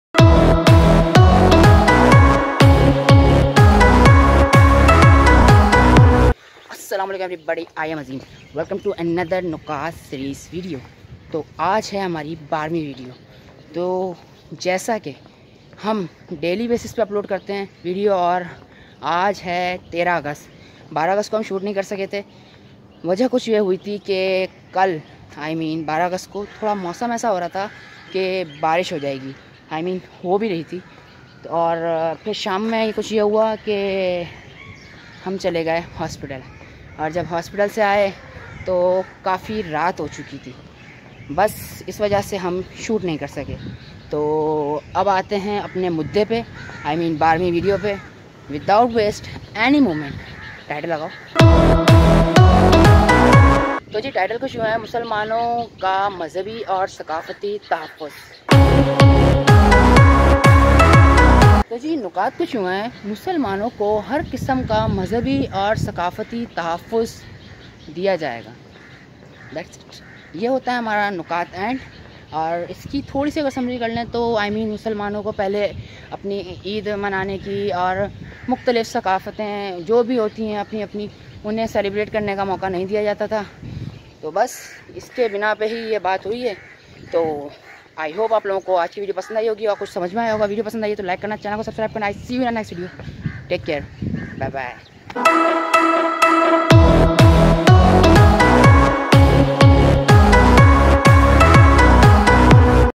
12th POINT | QUAID E AZAM 14 POINTS | AZEEM AHMED KHAN | WOW
Hi, my name is Azeem.

QUAID E AZAM 14 POINTS | 12th POINT
12th Point: The constitution should embody adequate safeguards for the protection of Muslim culture and for the protection and promotion of Muslim education, language, religion, personal laws and Muslim charitable.